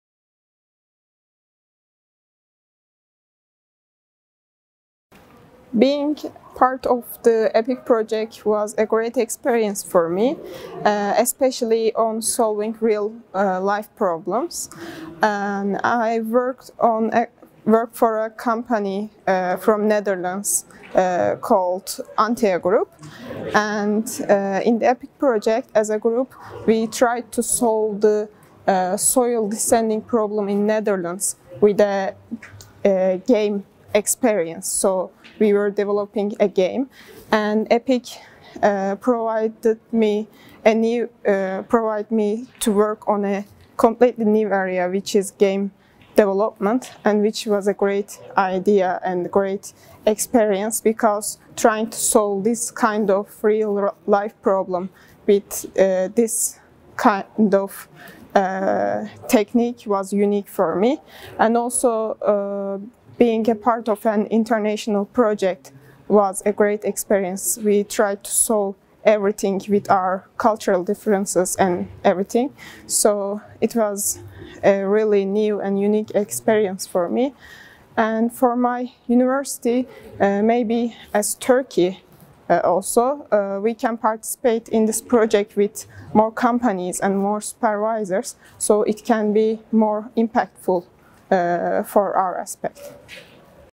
Hatice Nur Agba about the EPIC collaboration
In this video, Hatice talks about her experience in the EPIC collaboration, and what her group was working on.